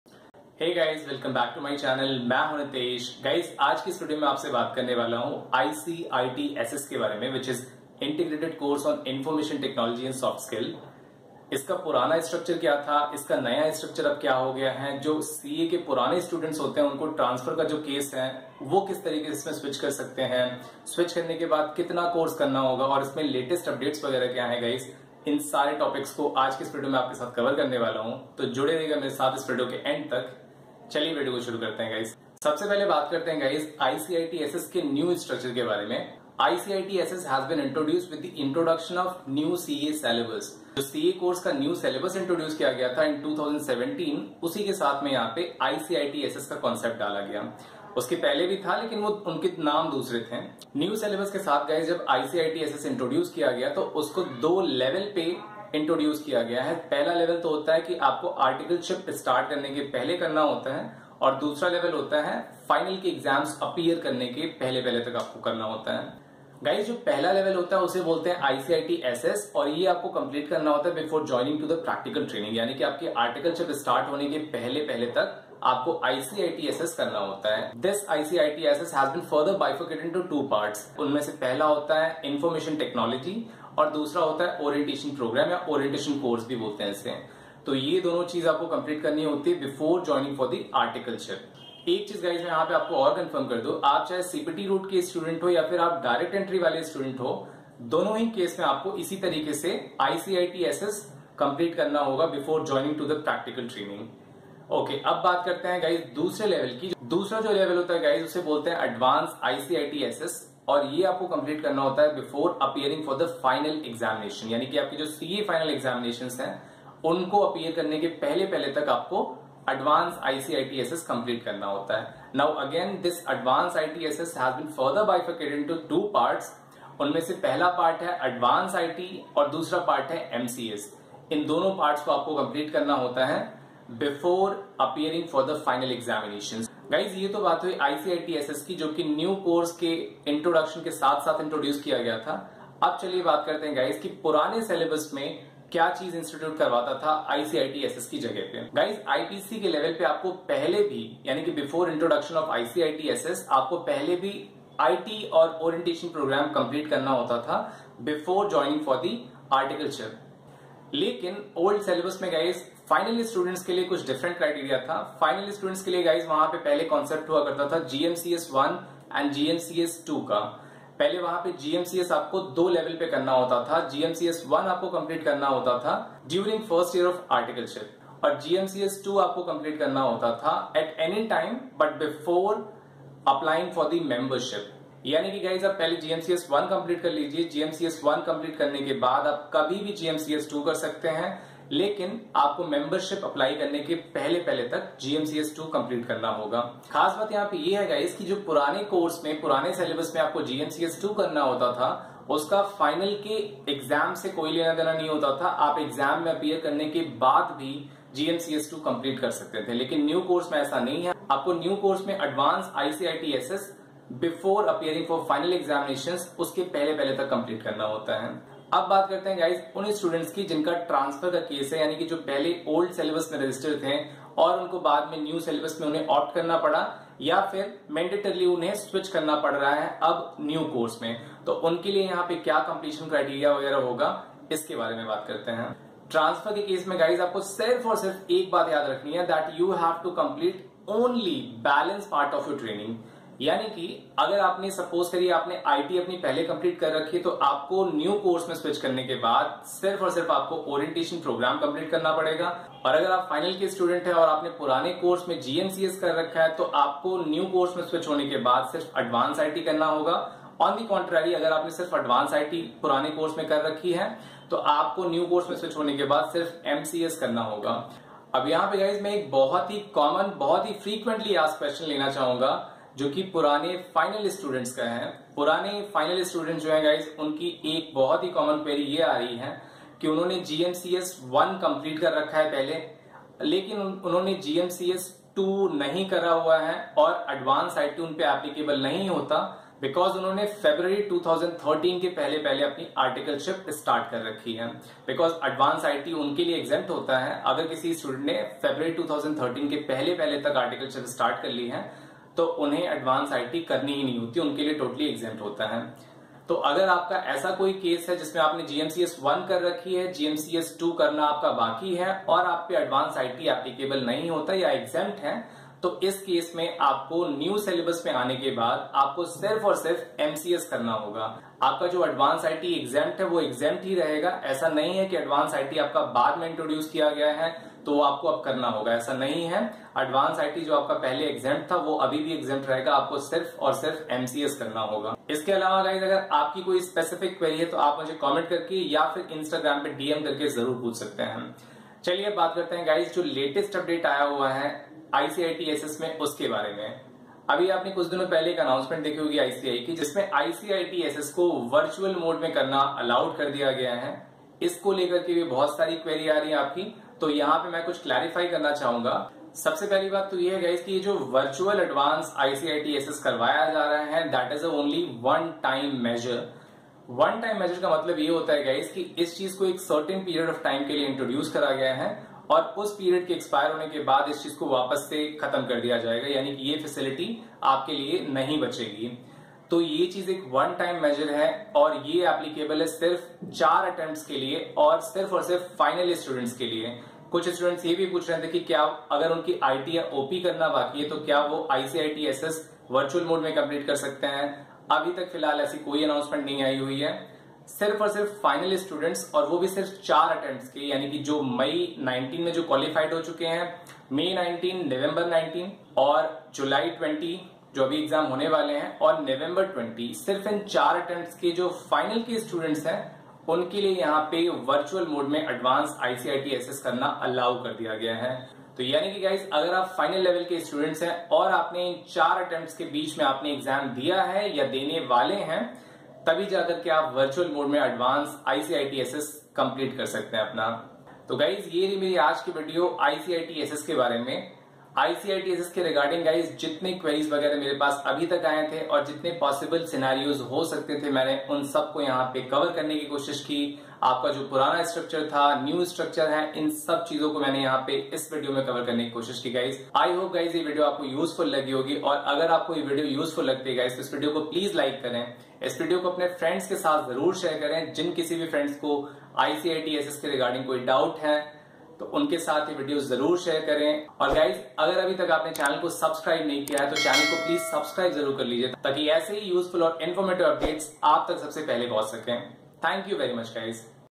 CA Exams 2020: Complete Details About ICITSS & Adv. ICITSS (Adv IT/MCS) | 30th May 2020 | CA Effects
Many Students are having doubts regarding the ICITSS & Advance ICITSS Courses. CA Students are confused about their applicability, their partial completion, and also about the latest updates regarding the Virtual Completion of Advance ICITSS (i.e. Virtual Adv IT & Virtual MCS). In this video, I have tried to cover every such doubt and answer them. Please do watch and share this important video.

In case of any query or suggestion, please do COMMENT  in the comment section below.